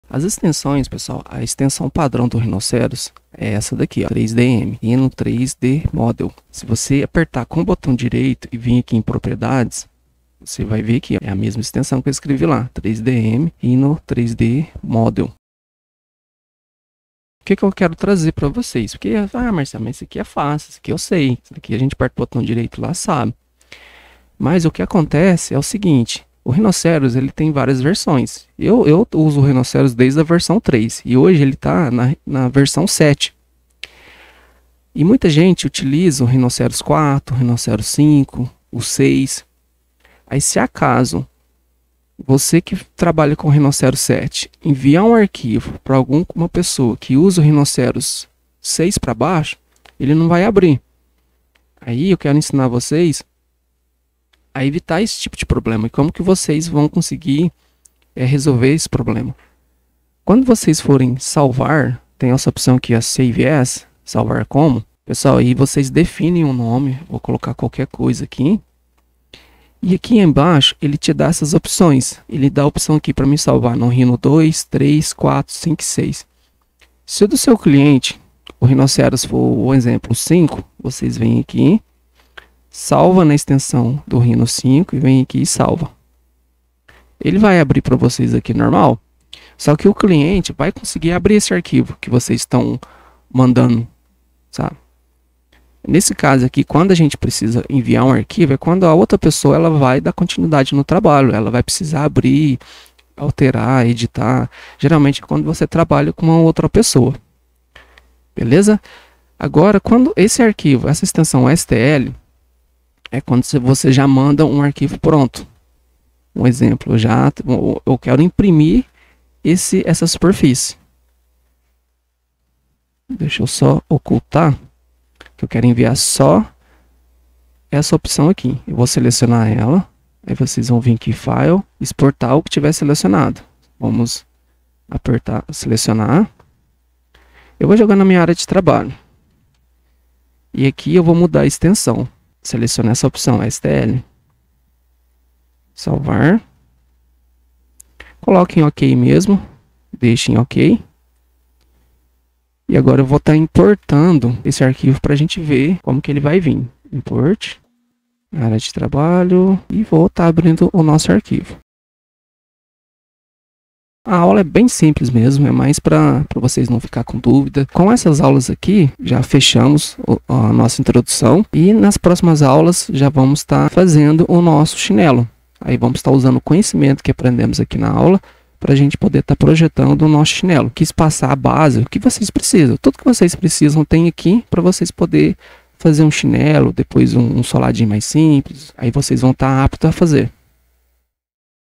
# 13 Trabalhando com Extensões do Rhino
Aprenda a utilizar as extensões do Rhinoceros.
Aprenda a salvar seu projeto para funcionar em versões mais antigas

Aprenda a exportar seu projeto para ser impresso por um impressora 3D